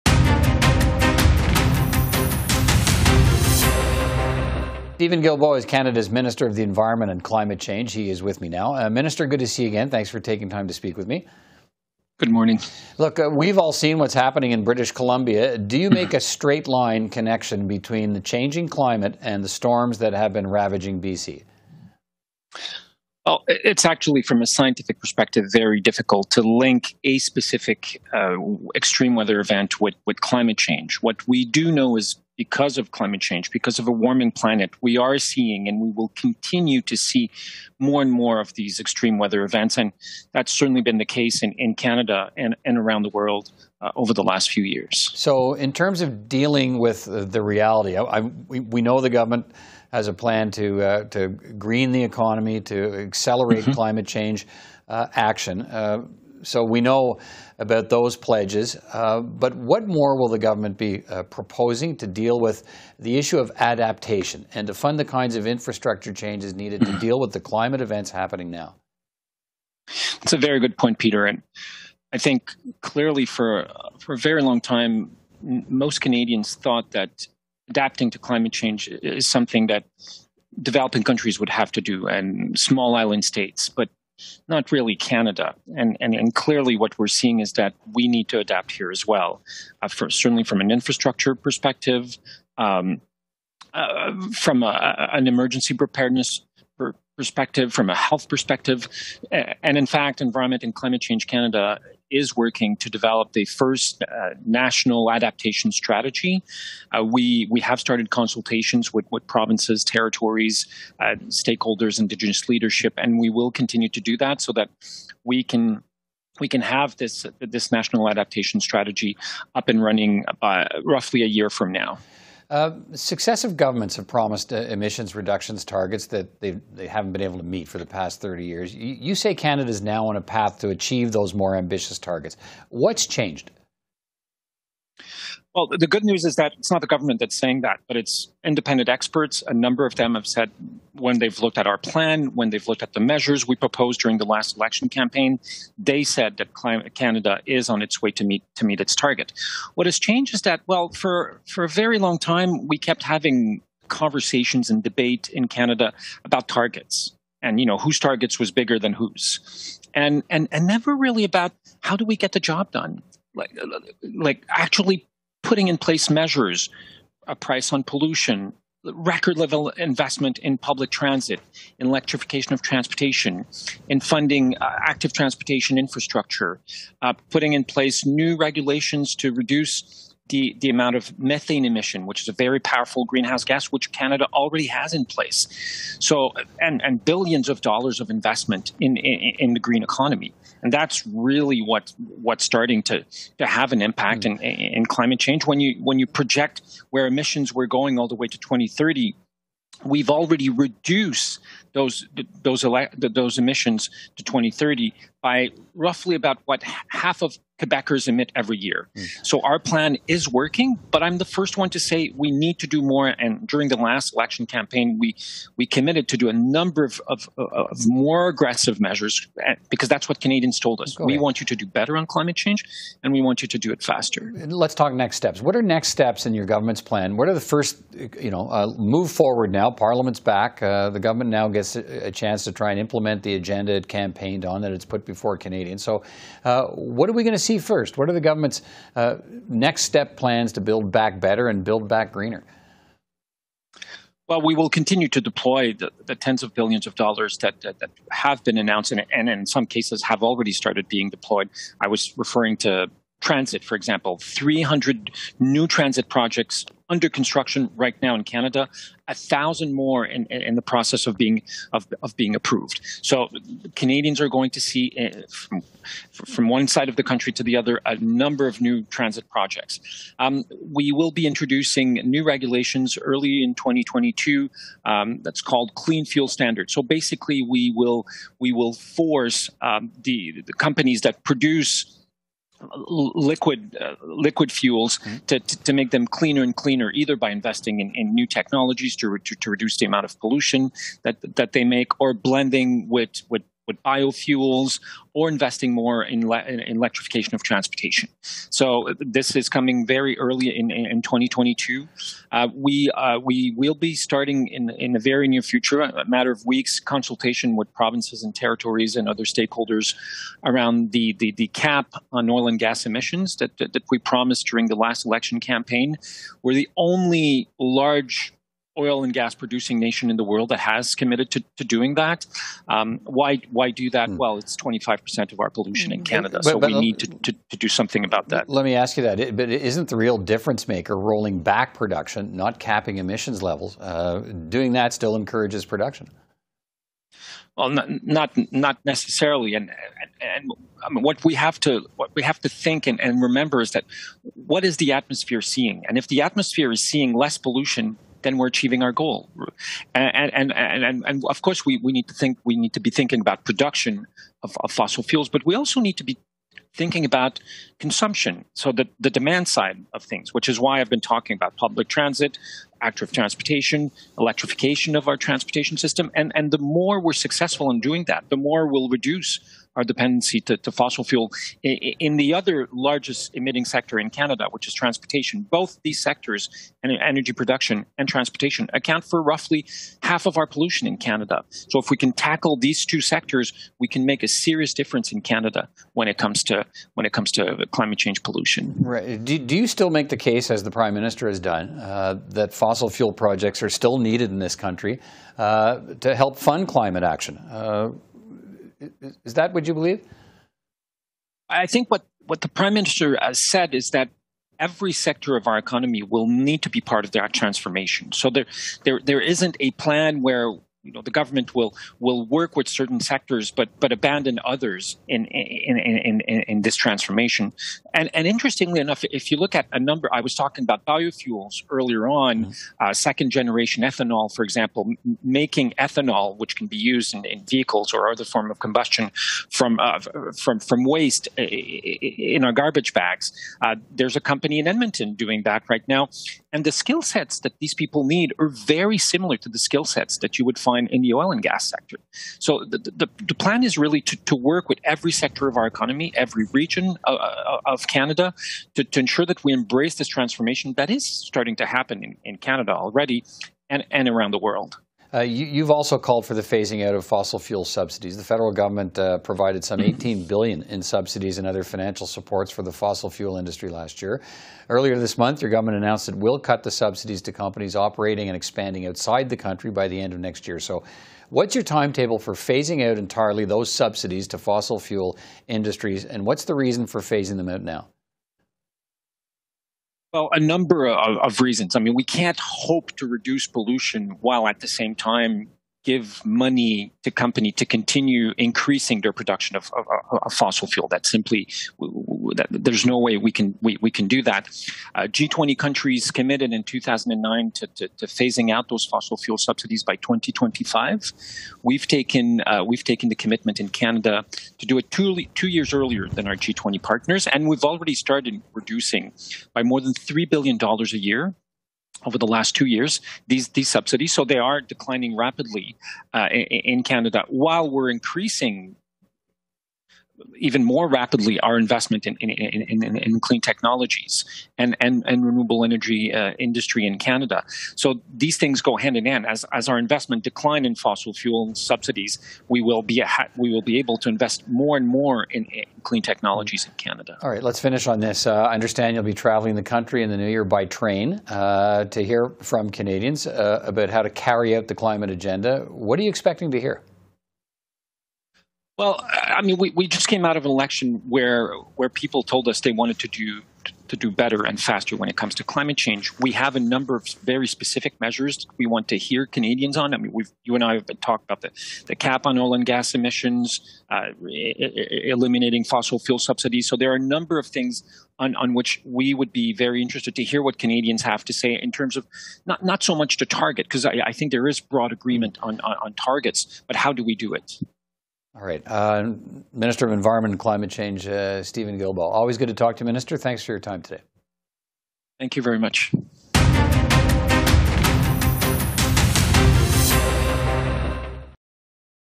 Environment and Climate Change Minister Steven Guilbeault on plan to address climate change
Environment and Climate Change Minister Steven Guilbeault speaks with CPAC’s Peter Van Dusen about the extreme weather in B.C. and how the Liberal government plans to address climate change.